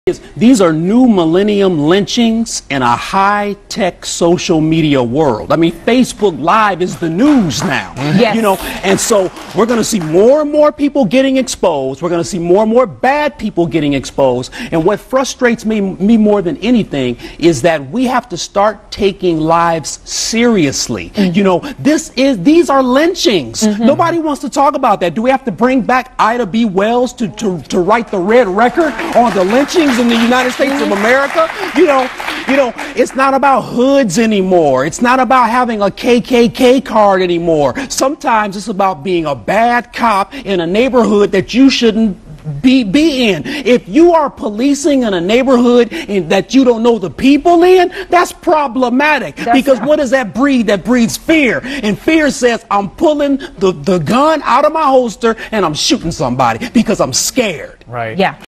Garrard McClendon on black men killed by police
Garrard McClendon discusses lynchings in new millennium.